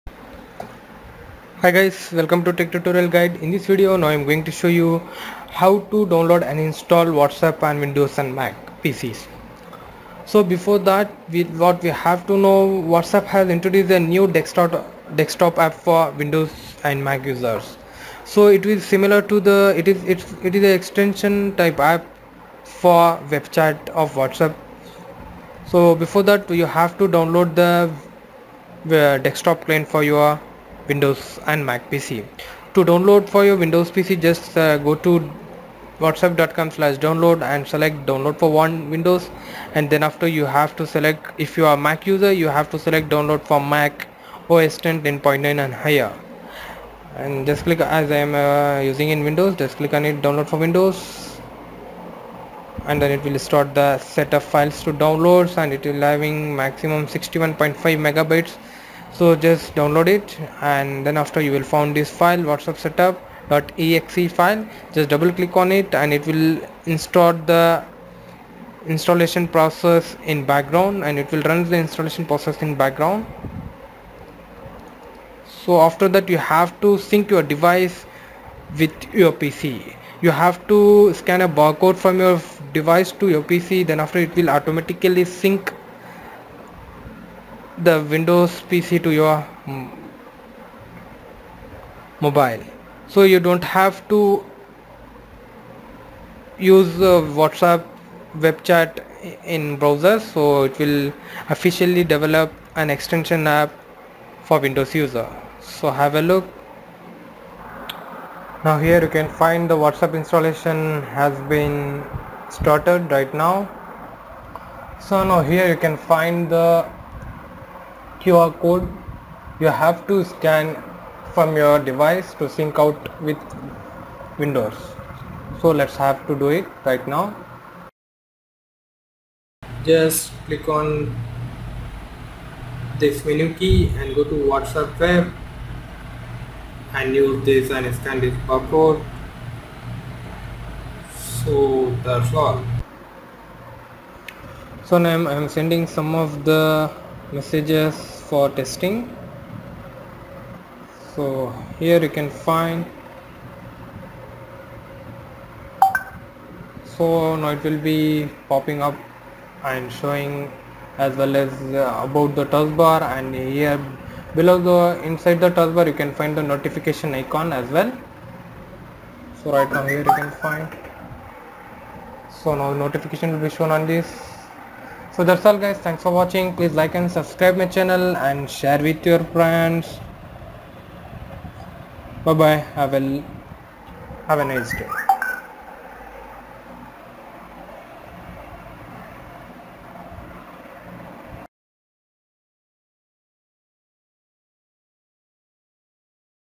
Official Whatsapp App For Desktop PC | Windows & Mac
Whatsapp App For Desktop 2016. Whatsapp has released a new desktop app for windows and mac..Download links are available down below.

TechTutorialguide